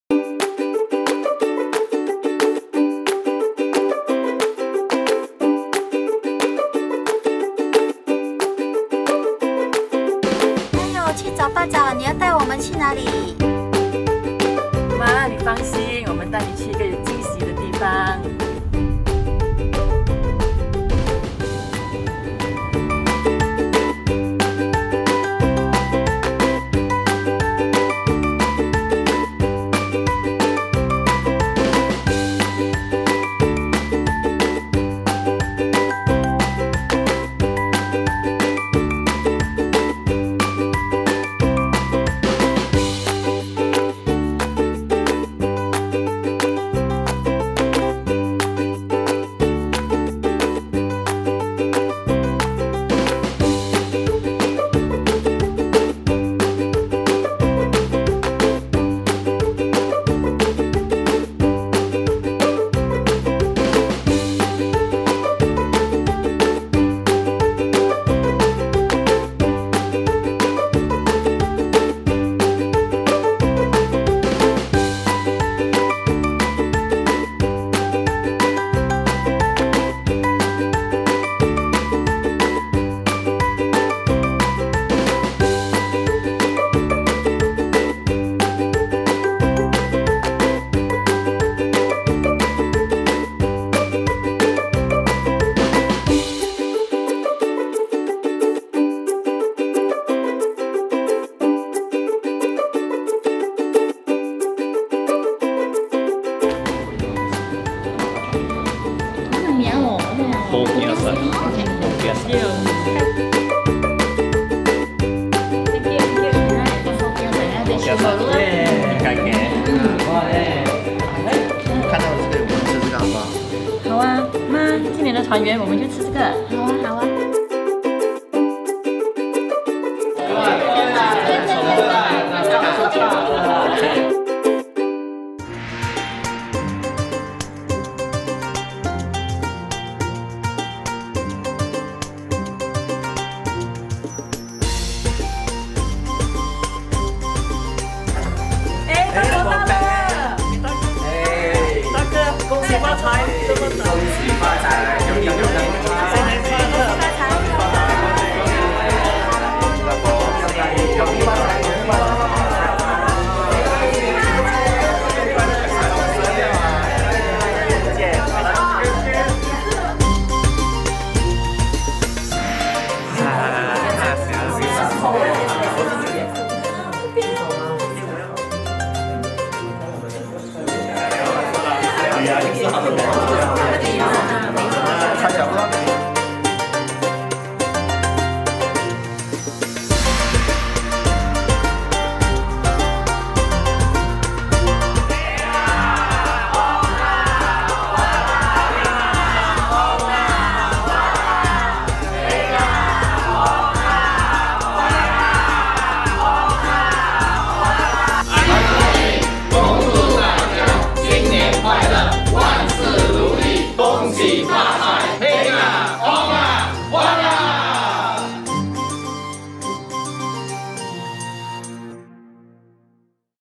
Iconic Group CNY Shortfilm 2019
Look at where this family heading to for CNY?

In collaboration with Iconic Development and Iconic Hotel, presented to you CNY Short Film 2019.

Iconic wishing you a Happy Chinese New Year 2019.

兴！旺！发！
Heng! Ong! Huat!